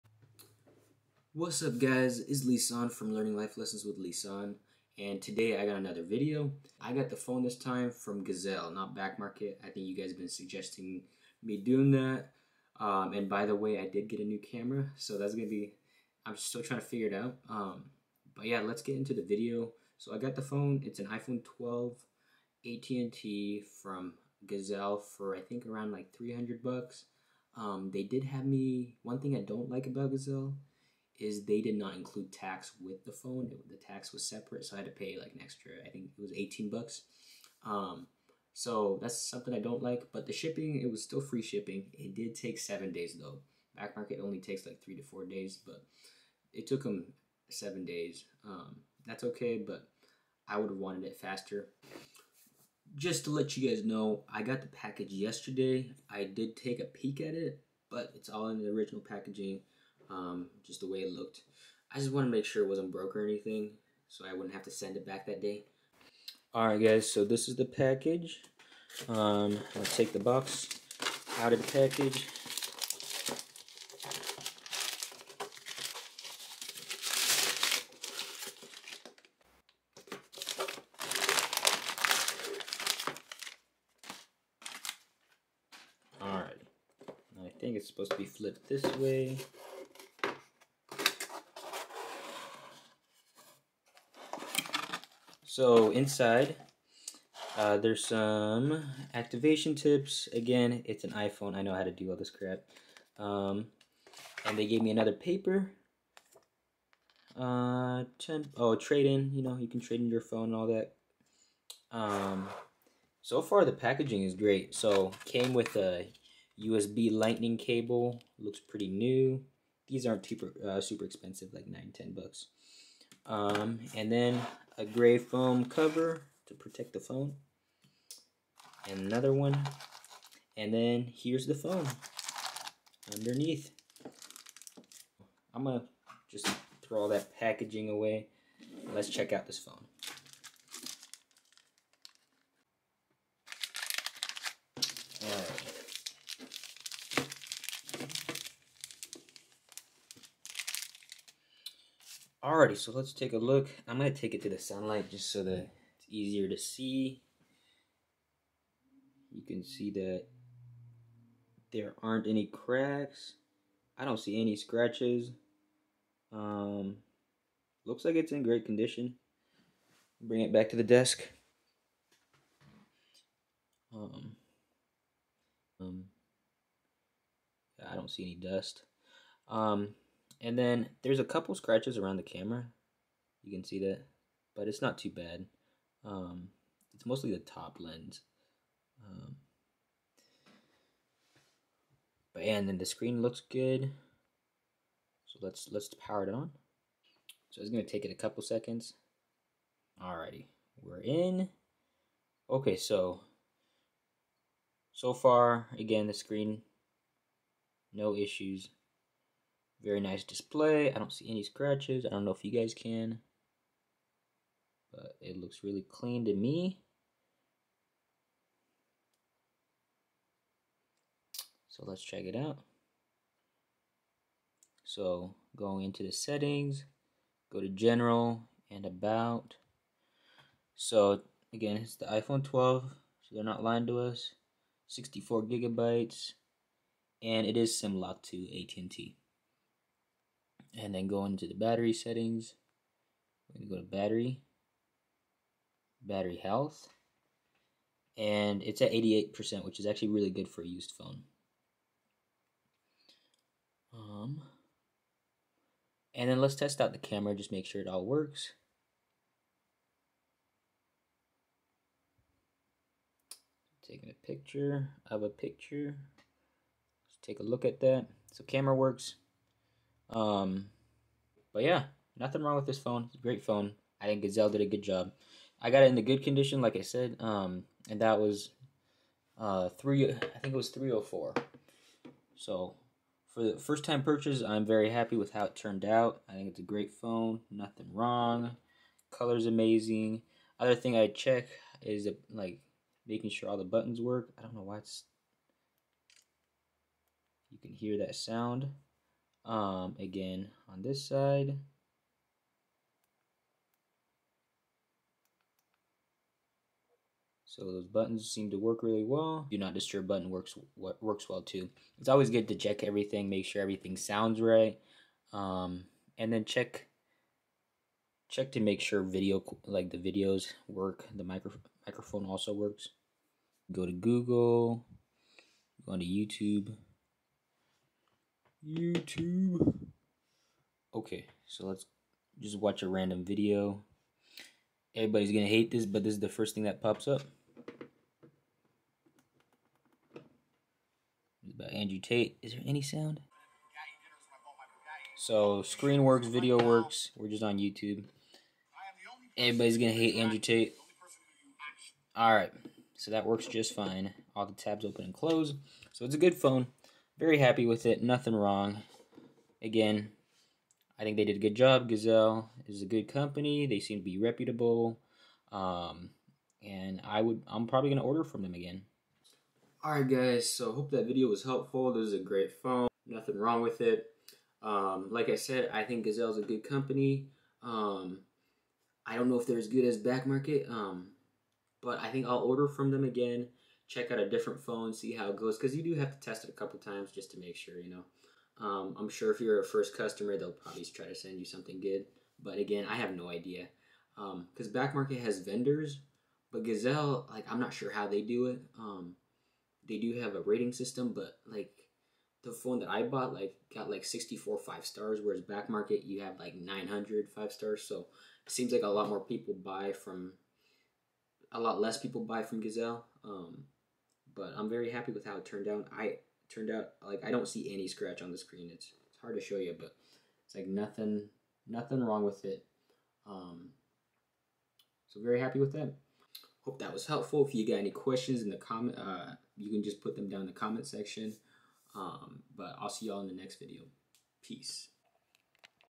Gazelle iPhone 12 Review and Unboxing
Hey Guys, this time I am reviewing the iPhone 12  that I got through Gazelle and will be telling you the positives and negatives of buying it from Gazelle. I highly encourage people to start buying used products and save themselves some money and not make payments!!